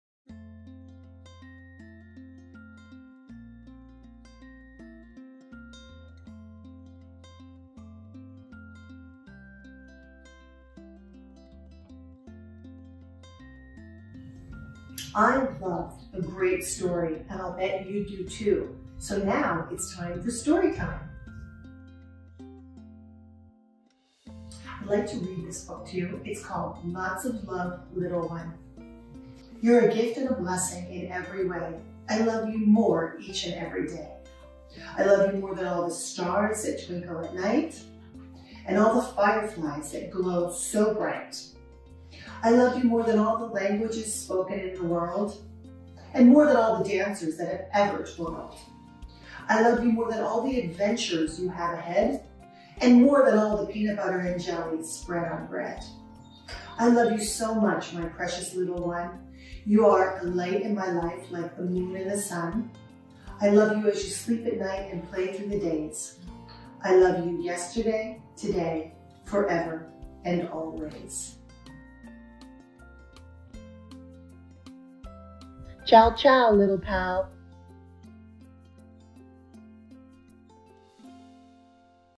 Hello You with Sandra Magsamen: Lots of Love Little One Storytime!
Hello You! You are a gift and a blessing in every way! Join bestselling author and illustrator Sandra Magsaman for a special reading of her picture book, Lots of Love Little One, and discover how much you are loved.  Let’s read, little one!